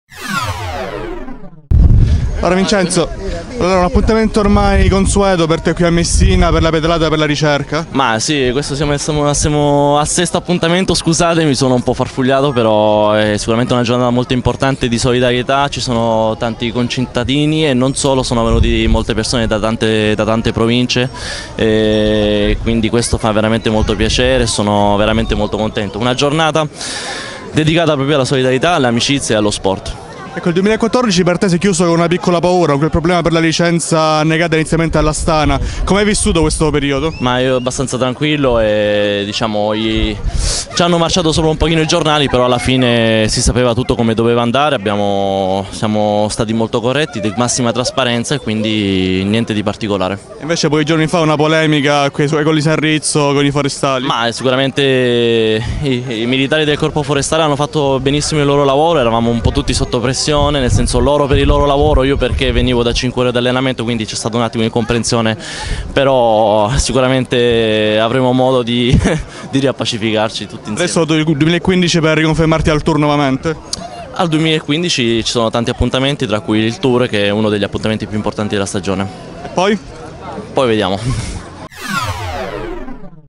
Vincenzo Nibali a Messina: "Forestale? Hanno fatto il loro lavoro"
Si è svolta questa mattina, come da tradizione da ormai sei anni, la “Pedalata per la ricerca – Fermare la distrofia muscolare di Duchenne”.  Giunta alla sesta edizione, si tratta di una iniziativa solidale organizzata dall’Associazione Amici di Edy Onlus in collaborazione con il Fans Club Nibali Messina Centro che si propone di raccogliere fondi per la ricerca: l'obiettivo è inoltre quello di lanciare un messaggio di sensibilizzazione su questa malattia genetica rara che anche in Italia colpisce diverse famiglie.  Ospite d'eccezione, il vincitore del Tour de France 2014, Vincenzo Nibali.
Intervista di Simone Intelisano
Riprese e montaggio di Francesco Minnella